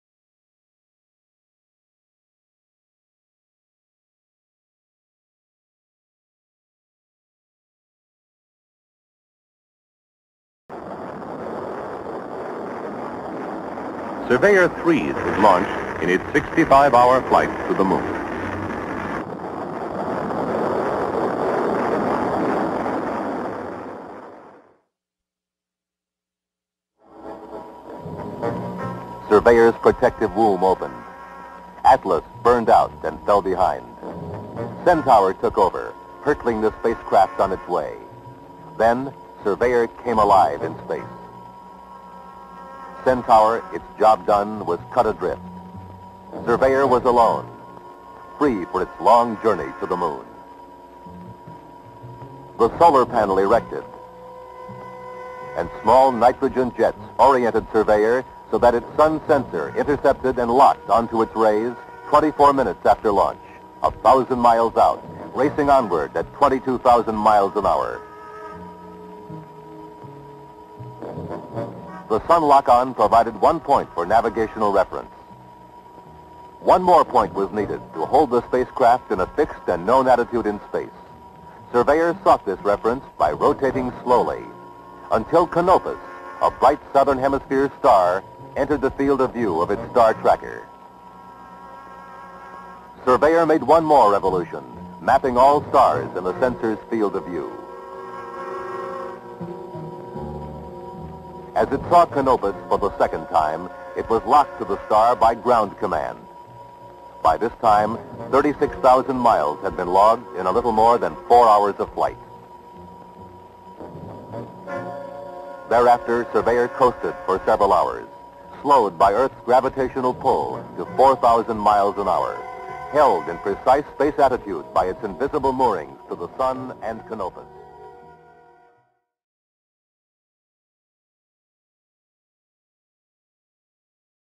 Launch of Surveyor III
The spacecraft Surveyor 3 is launched from Cape Kennedy, Florida, on April 17, 1967. It will become the second U.S. spacecraft to make a soft landing on the Moon, where it will study the lunar surface and send more than 6,300 pictures back to Earth.  Based on the spacecraft's surface sampling tests, scientists concluded the lunar surface was solid enough to hold the weight of an Apollo lunar module.